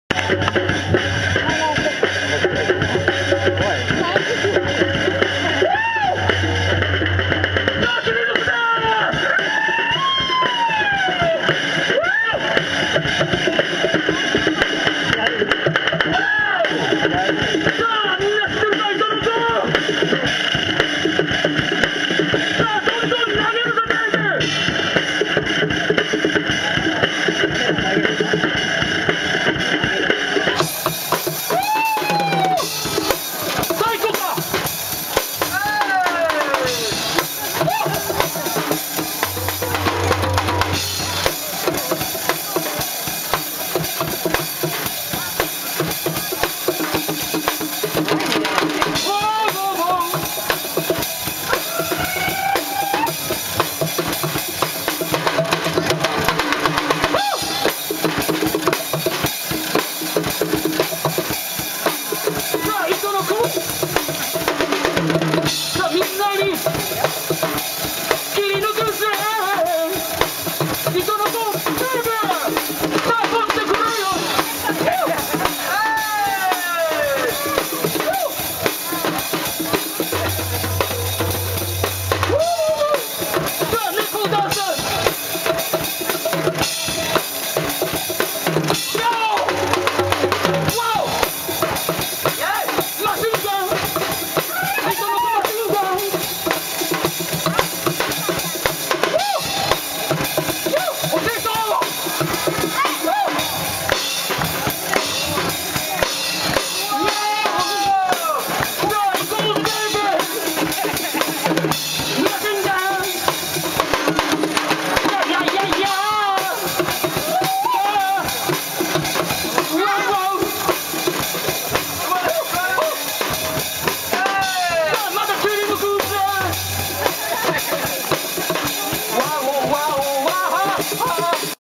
Itonokomachineguns~How to Use a Scroll Saw ~
Itonoko machineguns .It was and with the saw did. Japan, Asuke.
You should look at itonoko machineguns.
How to Use a Scroll Saw
糸鋸魔人が踊るきり抜く！！ゾントビがリズム刻む、踊る！！ターニングの熱いビート！！